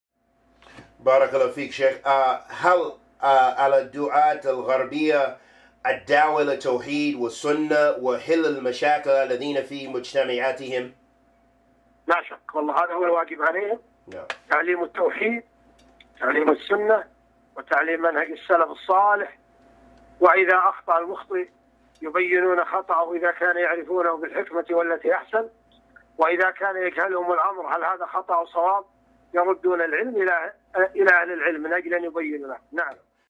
What Should Western Duat Be Doing? Sheikh Sa'eed Bin Halayl
Students of knowledge in their various societies should busy the people with that which is relevant to their society along with the call to tawheed, creed, and sunna.  سعيد بن هليل العمر
Question:
Is it upon the callers to Islam in the West to call to tawheed, and Sunna, and dealing with the issues they face in their societies?

No doubt that is what is an obligation upon them: teaching tawheed, Sunna, and the methodology of the Salaf and if someone makes a mistake clarify his error with wisdom and good speech and if he does not understand his error then return his affair to the people of knowledge so they can clarify for him.